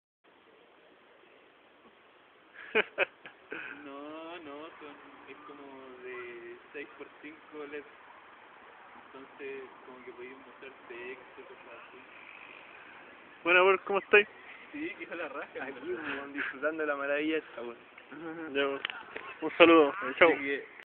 Captura pared de leds
Captura con mi super celu de la pared de leds vista desde la plaza